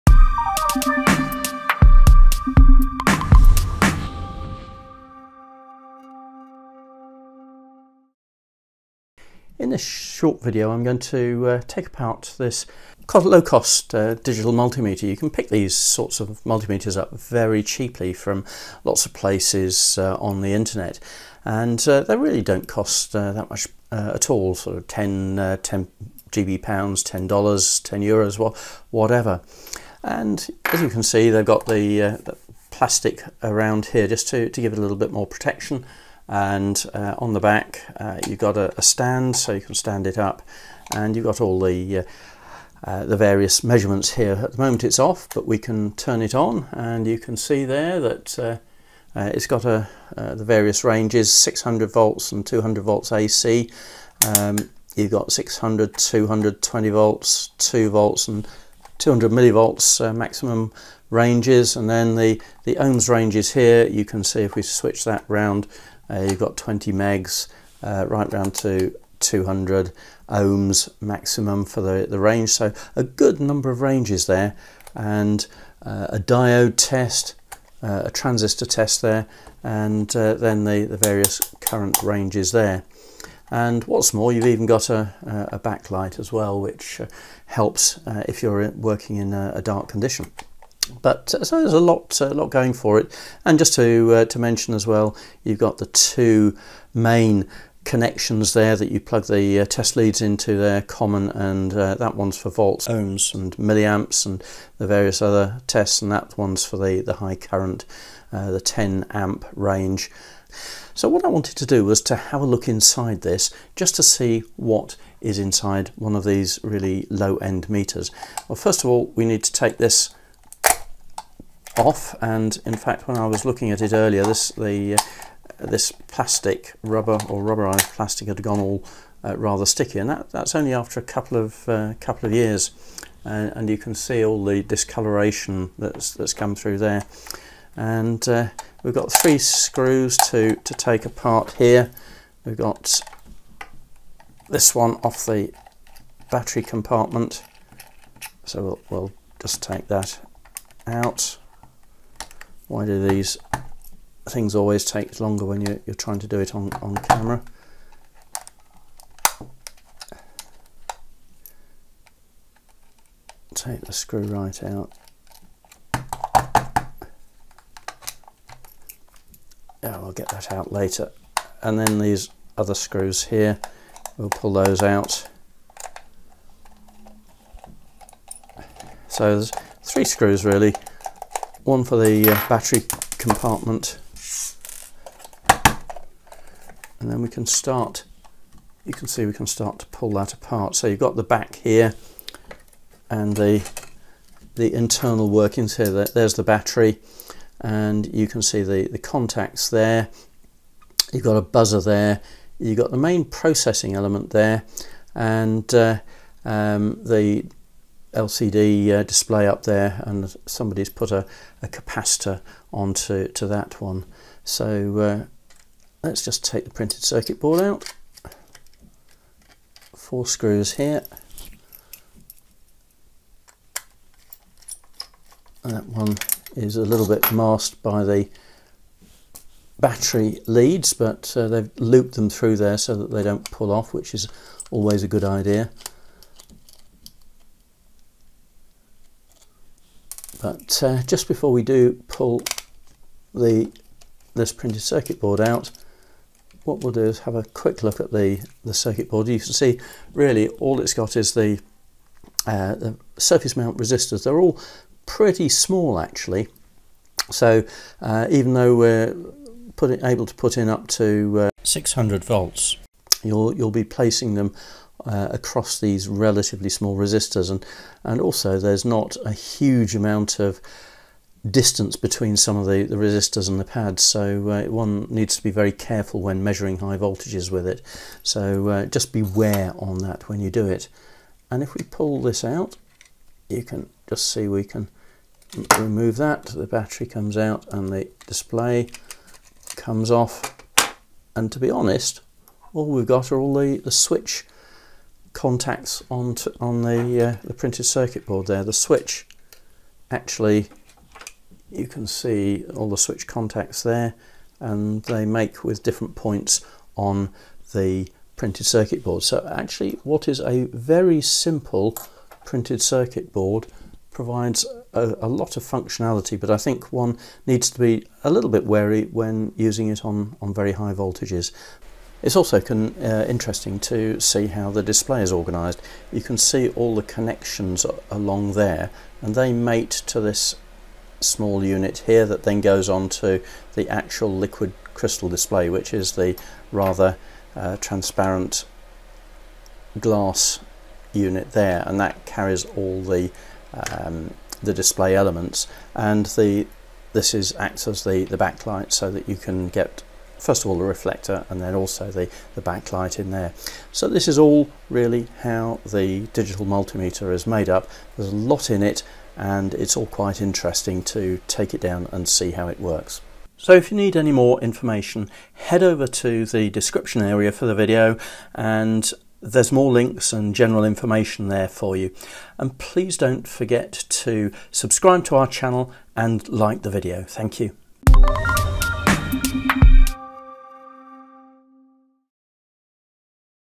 I Took Apart a $10 Digital Multimeter
It is interesting to find out what is inside digital multimeters or DMM. We ripped apart on of the low cost ones you can easily find not he Internet as it gives a good idea of what to look for.

In this video I take this DMM apart to look at the various elements inside it. You will learn lots about these instruments by watching and understand them far better.

The teardown shows the probe contacts leading to the overall printed circuit board and the various resistors that are on the board along with the main processor.

Although the board appears to be simple and straightforward it packs in a lot of functionality. The unit can measure volts up to 600V on both DC and AC, although there are far more DC ranges. It measures DC current from a whopping 10 amps down to 200 microamps full scale and there are also ranges for diode and transistor measurements.

It was noticed that even for the high voltage ranges the small surface mount resistors are used,. They are often specified to these high voltages, but I am a little wary about subjecting them to some of the higher voltages, and current levels that the meter can carry.

The liquid crystal display is also interesting. Connected via pads on the main PCB and there is also a reflector and backlight.

Overall a very compact unit which gives a lot of functionality for a very small cost//For more information about digital multimeters